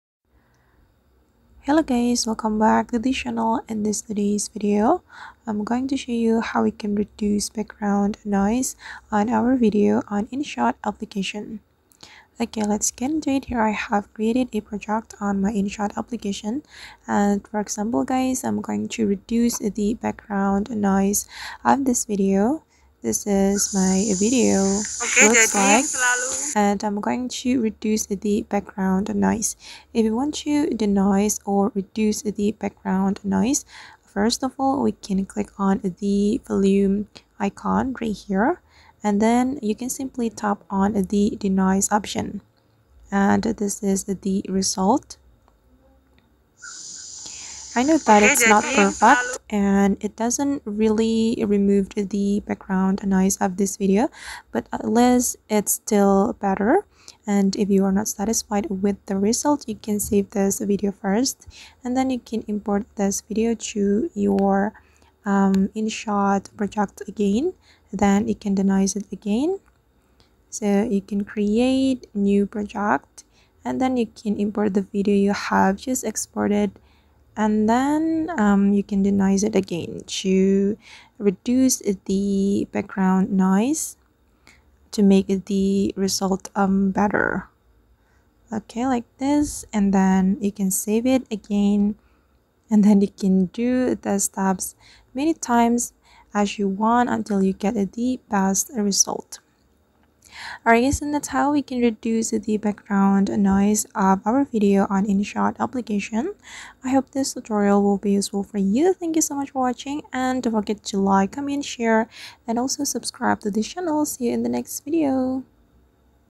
❤ ULTIMATE GUIDE: How to Reduce Background Noise of a Video on InShot
Are you tired of distracting background noise ruining your video recordings? Say goodbye to unwanted sounds with our ultimate guide to reducing background noise using InShot! In this tutorial, we'll walk you through step-by-step instructions on how to effectively eliminate background noise from your videos, giving your content a professional polish. Whether you're filming vlogs, interviews, or DIY tutorials, mastering this skill will take your videos to the next level.